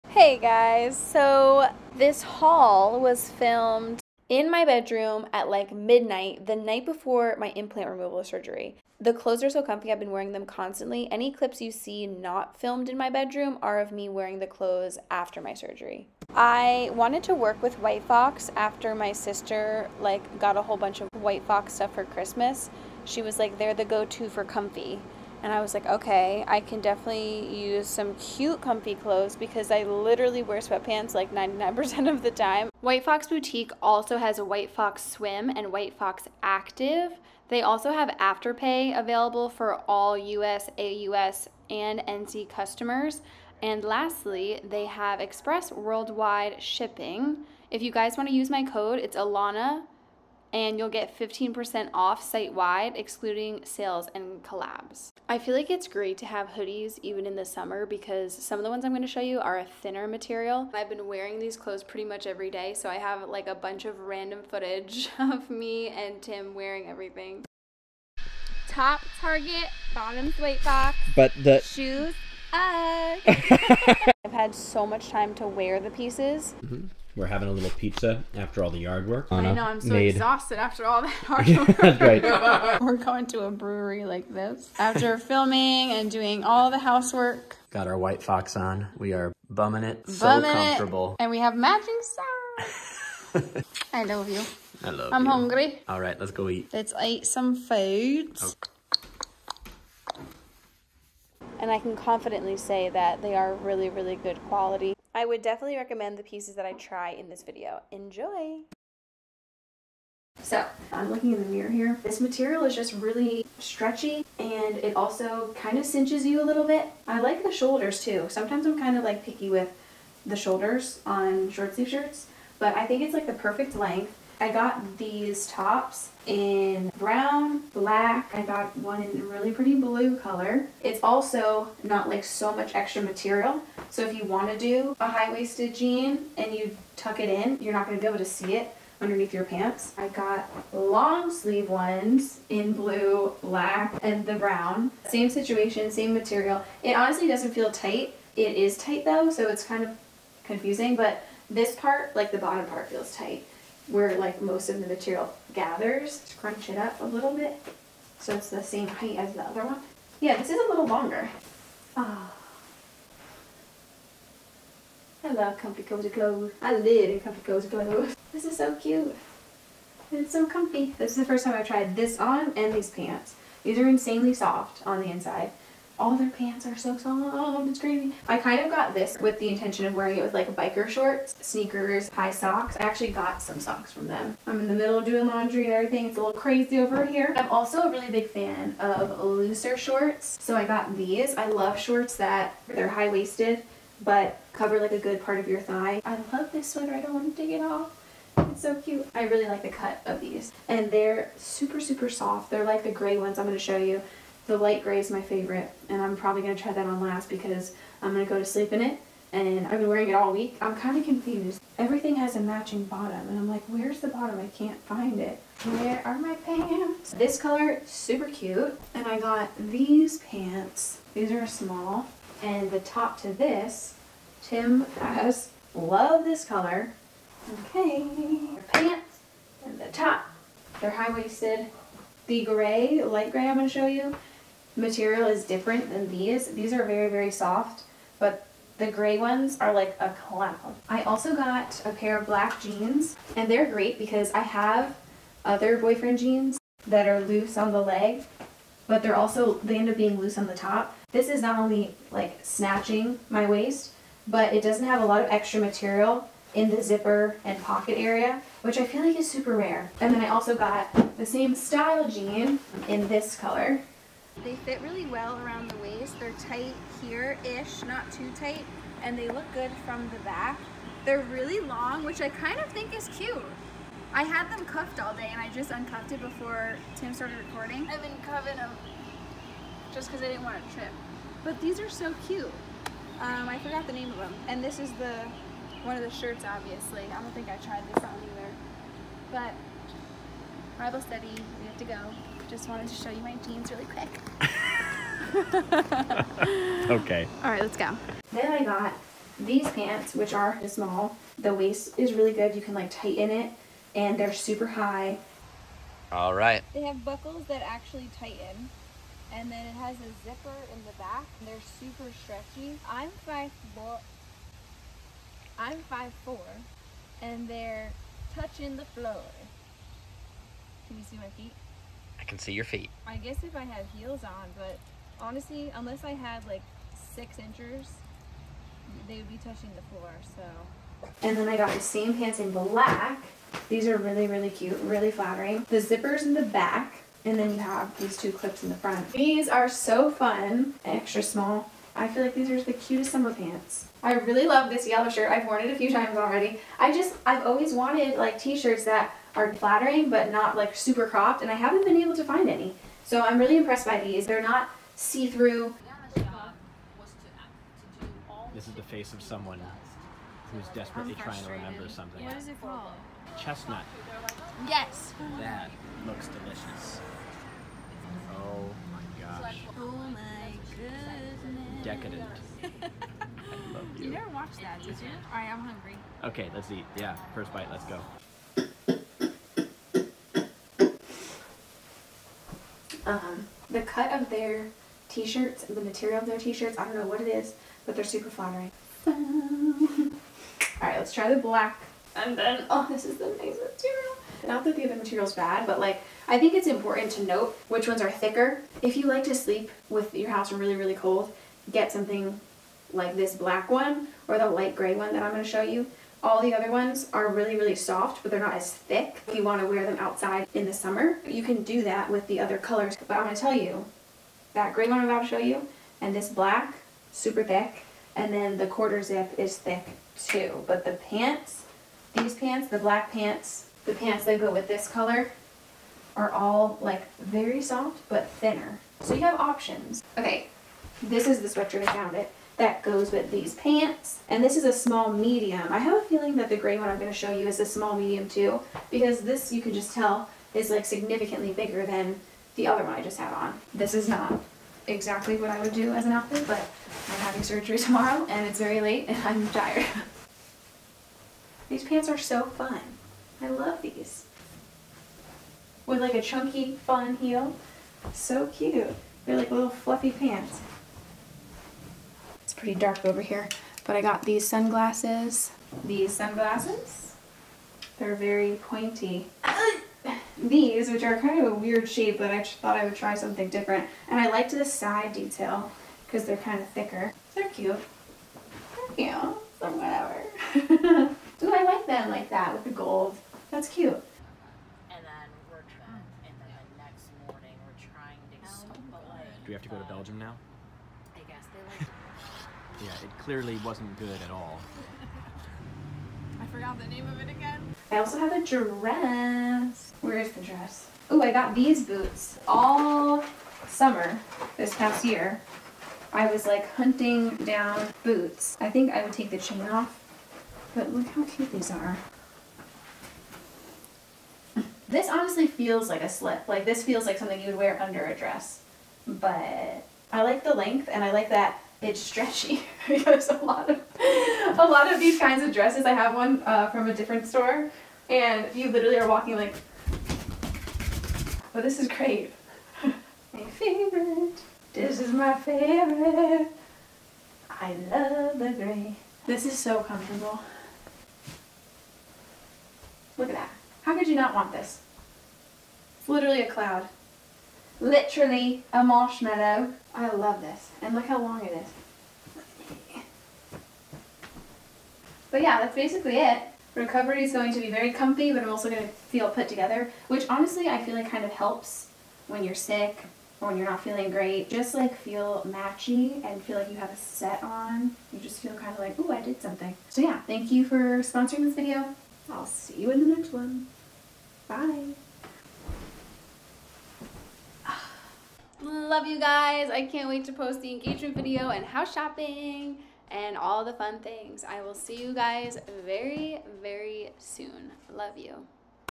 A classic try on haul mixed with random clips of things I've been up to =) ft. white fox
Products Mentioned:
Too Blessed Tee Mocha
Who's That Girl Straight Leg Denim Jeans Vintage Black